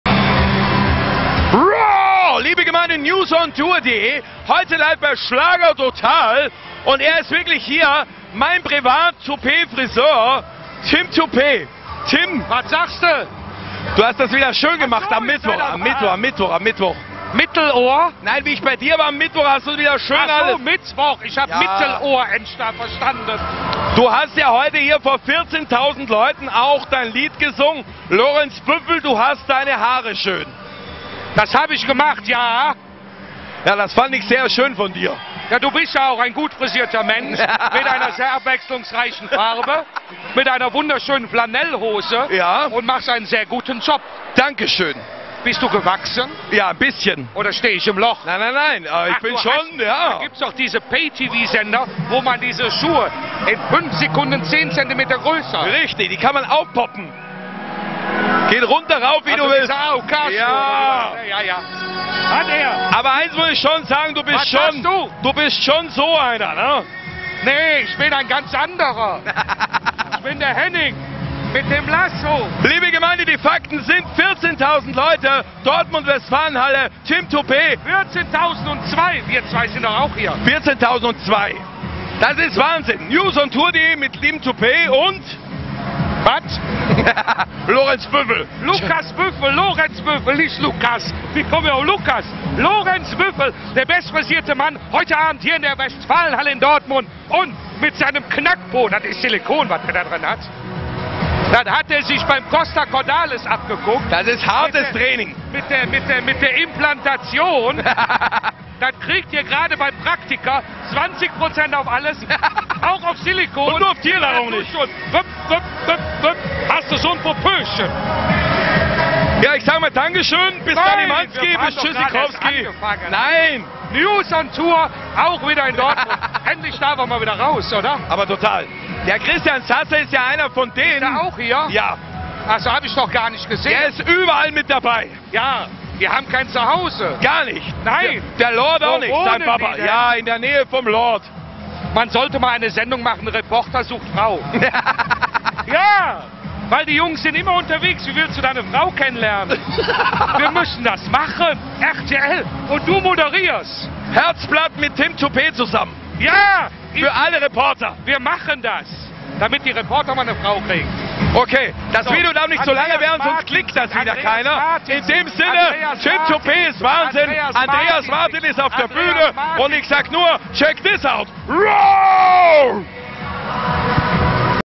Schlager Total 2010: Tim Toupet und Lorenz Büffel (Fun Interview) TV.NEWS-on-Tour.de
Schlager Total 2010: Sänger Tim Toupet wird von Lorenz Büffel in einem "Fun Interview" zur rede gestellt...

Produziert am So. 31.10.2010 für
@Dortmund, Westfalenhalle

Nicht freigegeben für fremde Medien!!!
Einbinden oder ausstrahlen nur mit spezieller Genemigung von erlaubt!!!

Quelle: YouTube reicht DEFINITIV nicht aus!!!